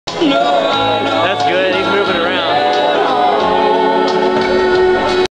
Ralph Fisher and armadillo
This video shows a few seconds of slack time between armadillo races. The owner, Ralph Fisher is shown holding Phylis Dillo, the tamest of the racers.